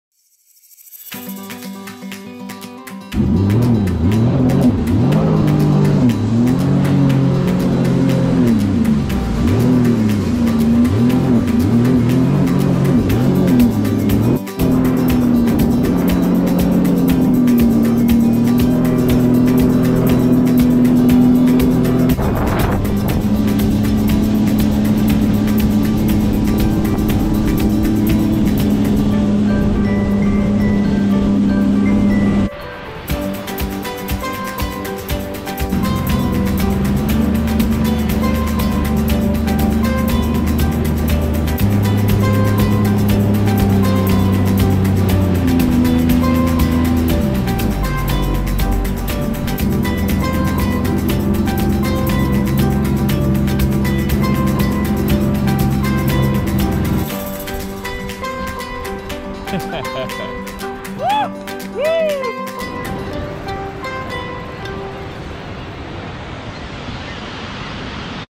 10 Glacier Tour in Prince William Sound
A great day exploring the glacier waters in PWS back in July, 2020